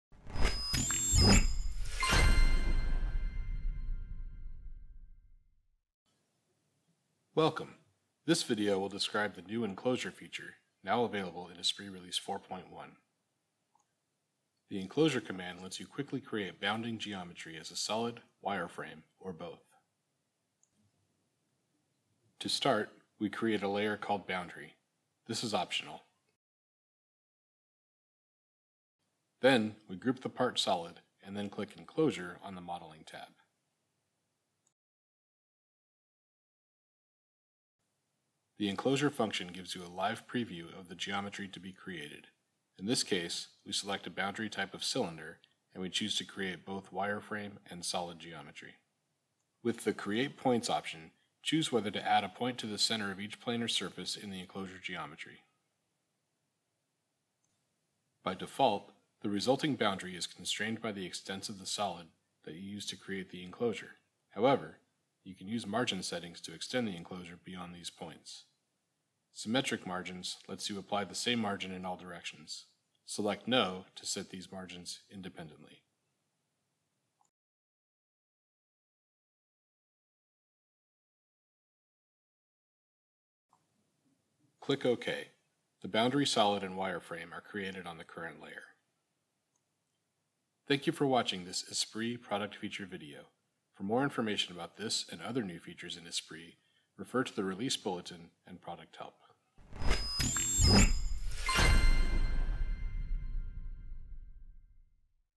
ESPRIT® Product Highlights: Bounding Box
Use the new Enclosure command on the Modeling tab to quickly create bounding geometry as a wireframe and/or a solid. Choose a box or cylindrical shape for the enclosure, and optionally generate points at the center of each of the enclosure’s planar faces.